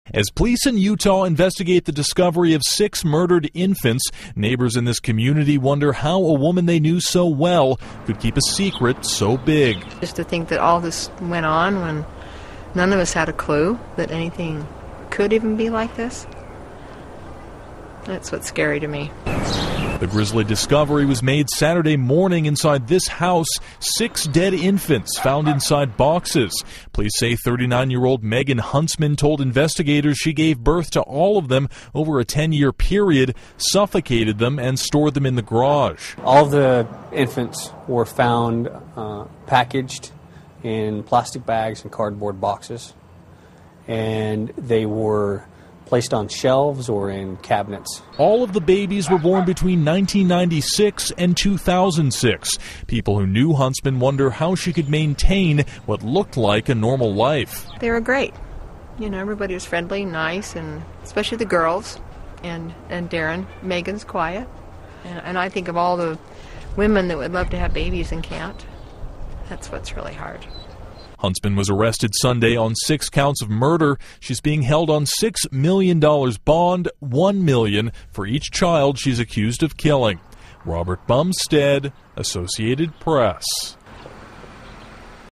Police: Woman Stored Dead Babies in Garage
A Utah woman is facing six counts of murder after six dead newborns were found stored in boxes inside the garage of her suburban home. (April 14)